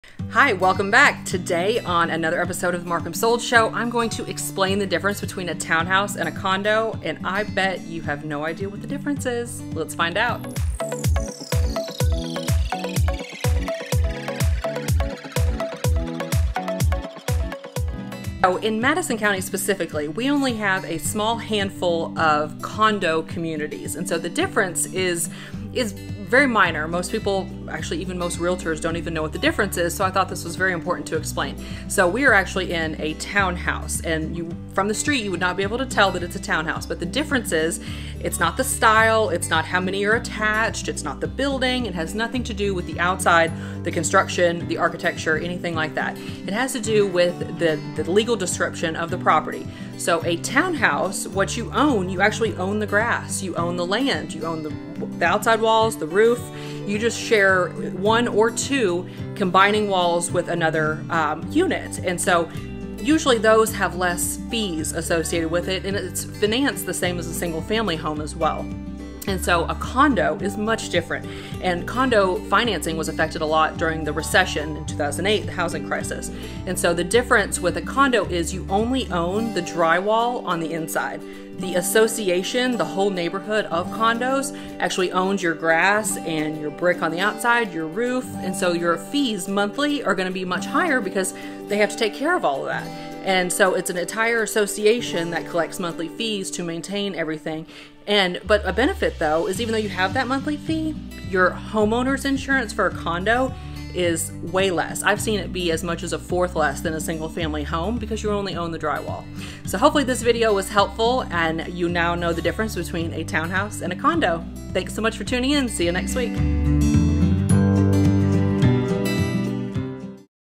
What's the Difference Between a Townhouse and a Condo?
On today’s MARCUMsold Show I’m giving you insight about what constitutes a condo vs a townhome. You’d be surprised at how many people (even realtors!) don’t actually know what the difference is. And I’ll give you a hint - it has nothing to do with the style, construction, design, or layout of the building. Check out this week’s video to finally, once and for all, know the real difference between condos and townhomes!